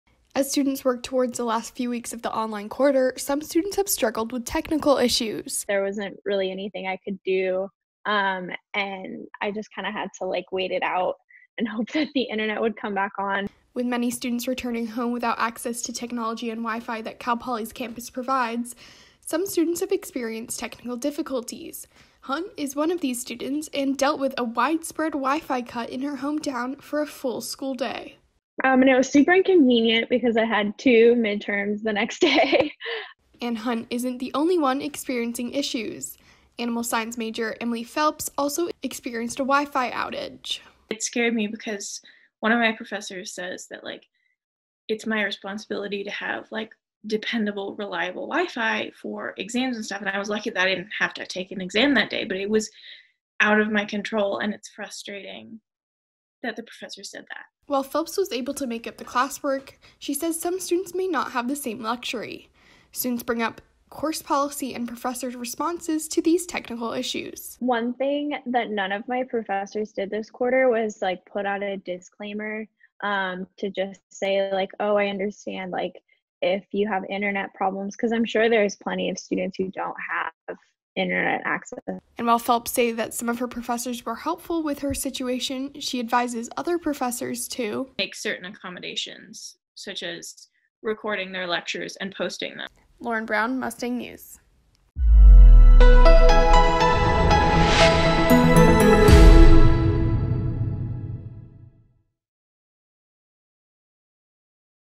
Students experiencing WiFi outages say it has severely impacted their studies
With many students returning home without access to the technology and WiFi Cal Poly's campus already provides, some students have experienced technical difficulties that have severely impacted their studies.